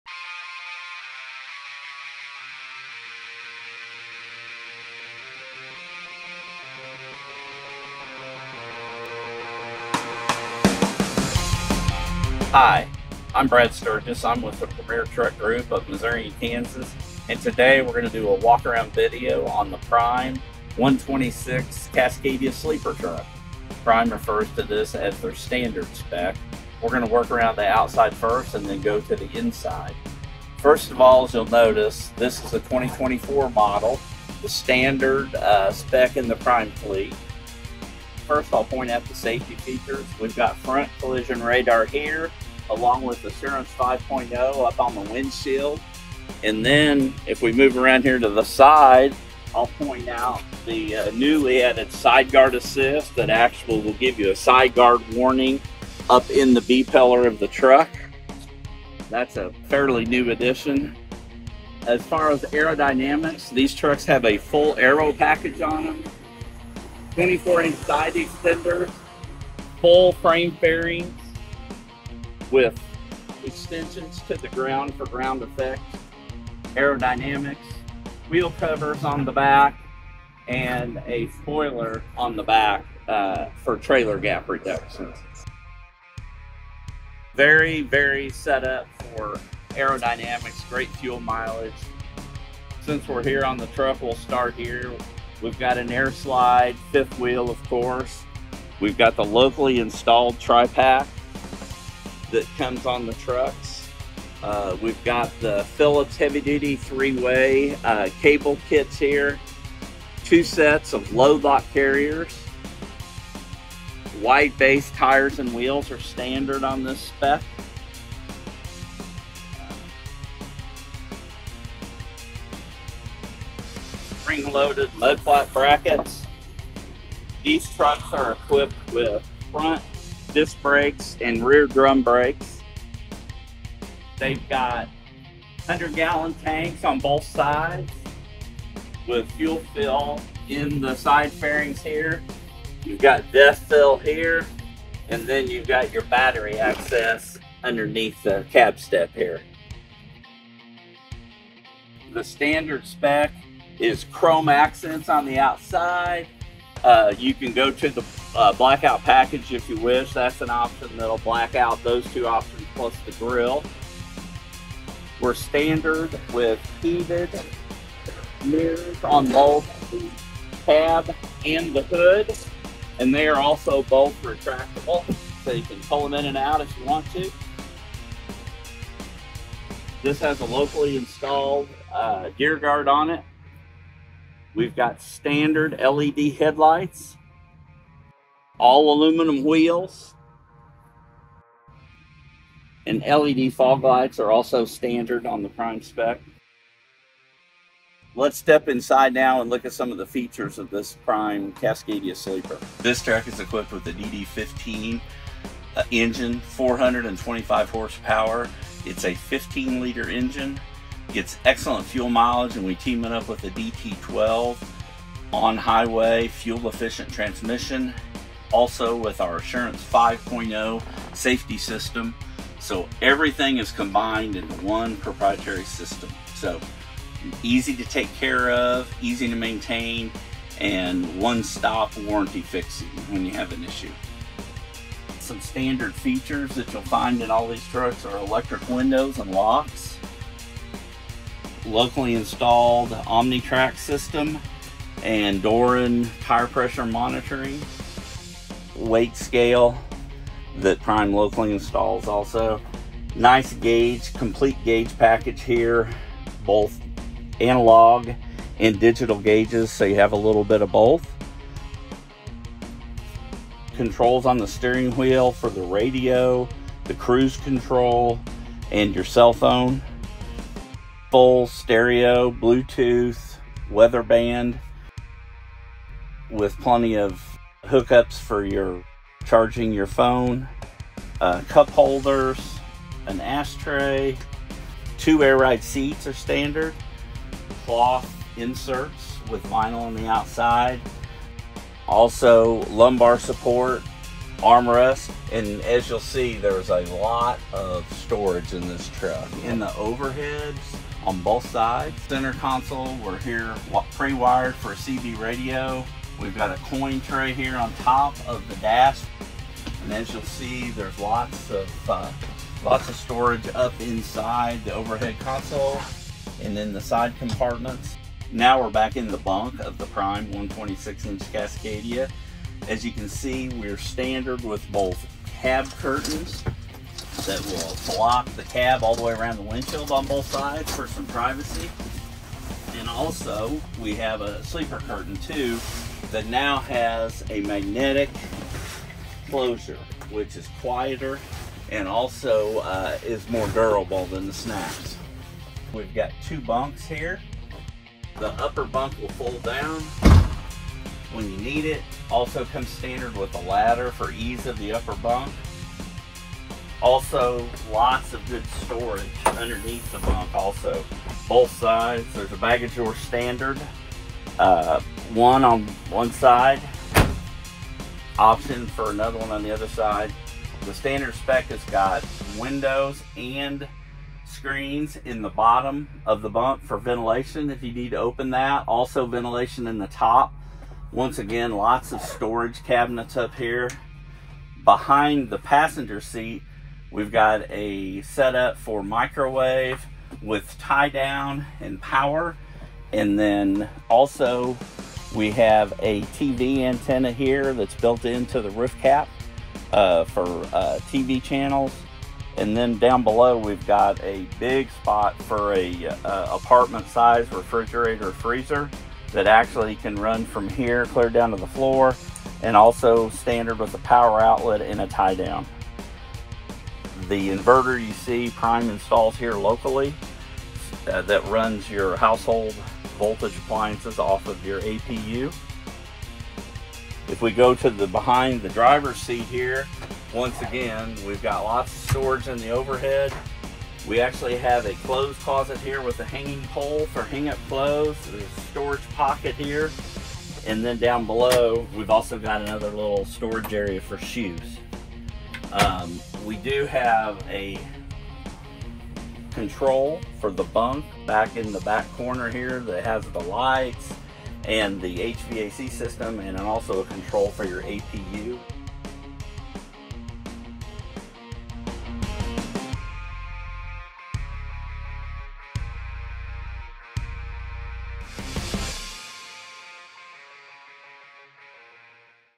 2024 Freightliner Truck Tour
Check out the latest Freightliner in this 2024 tour! Brad, our Freightliner rep shares the latest in the 2024 trucks with some added features such as Side Guard Assist.